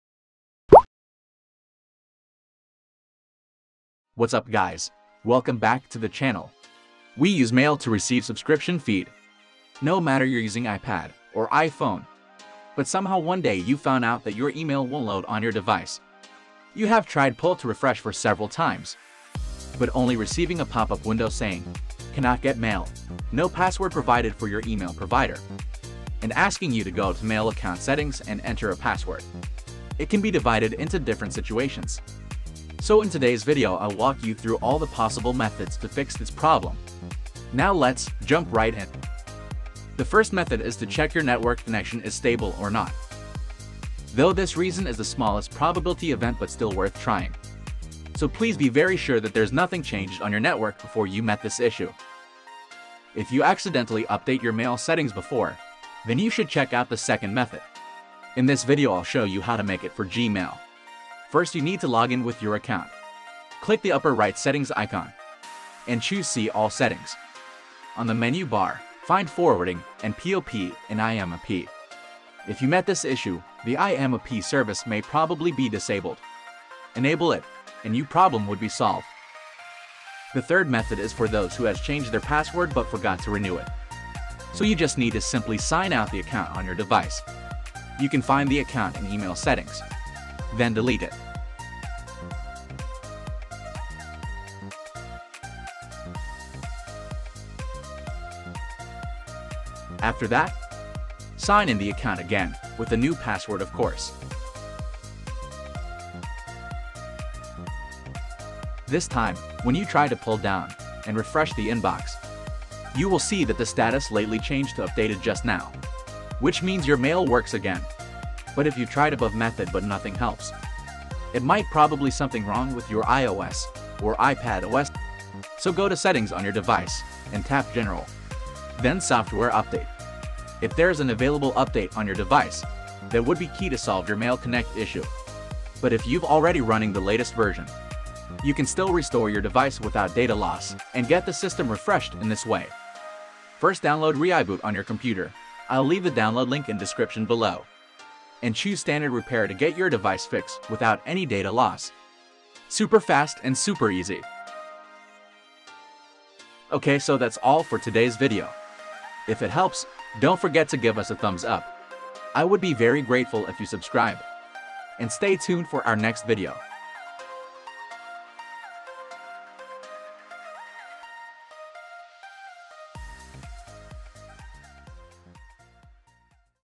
Cannot Get Mail on iPhone/iPad? Here is the Real Fix! 2022
If your device also shows cannot get mail, watch this video to get the latest solution. Check it out now

Timestamps:
00:00 Intro & Preview
00:43 1. Check Your Network Connection
00:56 2. Enable IMAP in Email Settings
01:22 3. Delete Account & Reload
02:04 4. Update iOS/iPadOS System
02:23 5. Restore Device without Data Loss